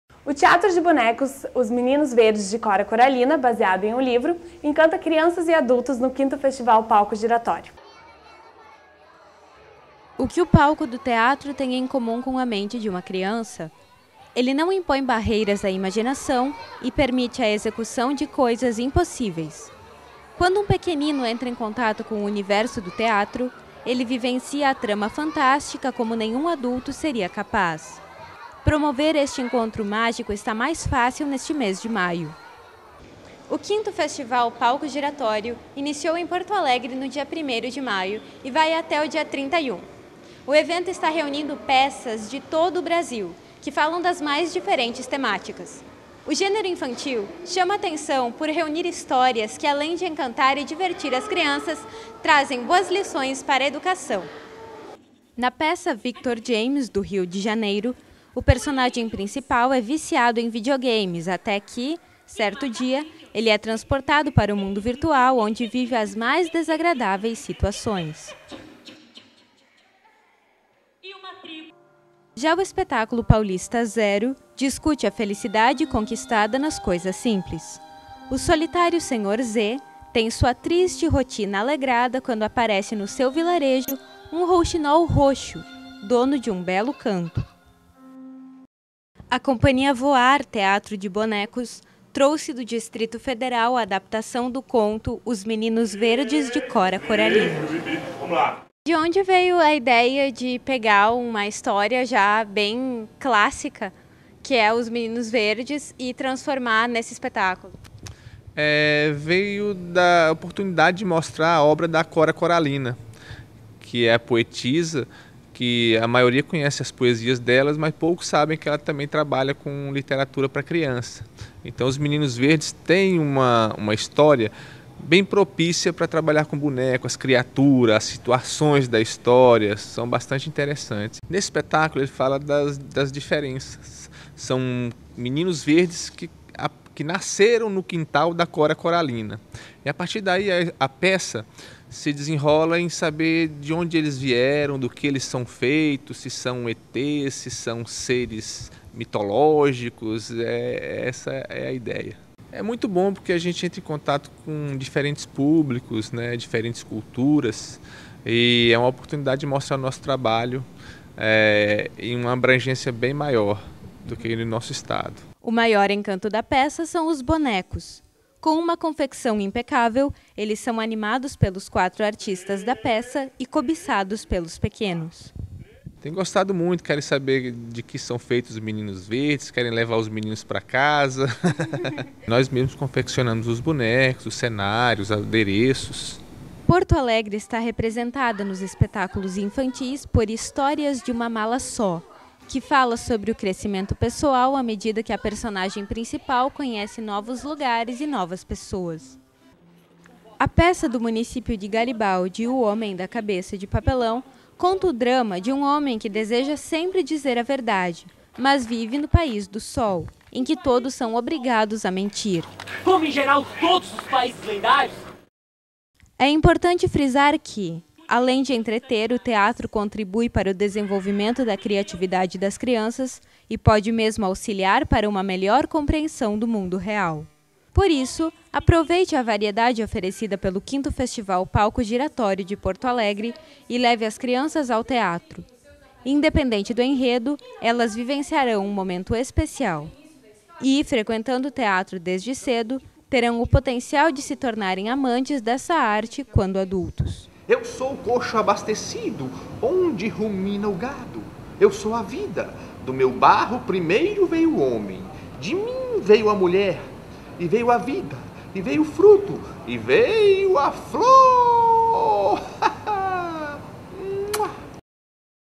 Caderno 2 - Peças infantis do Palco Giratório
Matéria sobre as peças infantis da quinta edição do Palco Giratório